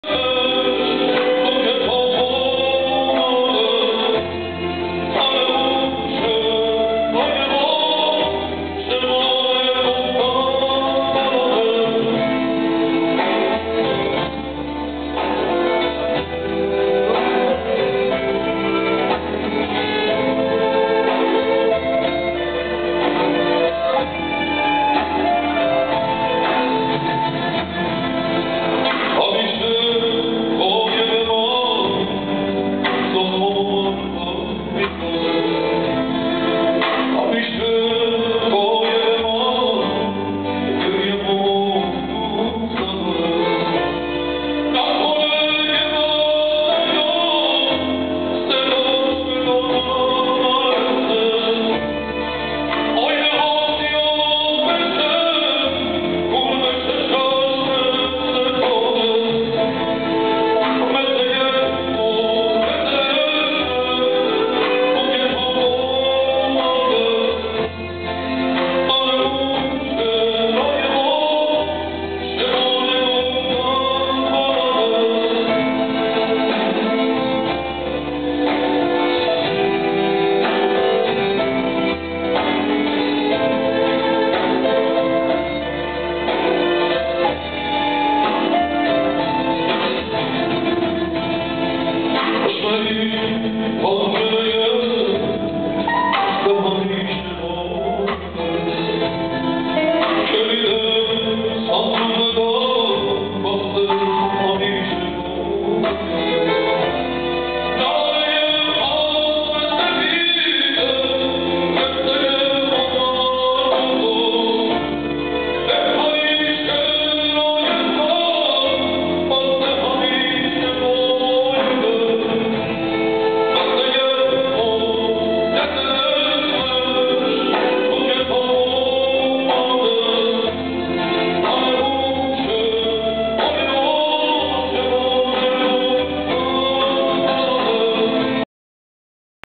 Michael Los Angeles NOV 6, 2010 Foroughi / Farhad Concert -- Saban Theatre -- PART 3
By far the best concert i've been to in past 12 years, Michael is one of a kind, I really felt I was in a foroughi/farhad concert. BTW this wasn't a comdey show, This was a real Concert and a great job by Michael, I can't say enough.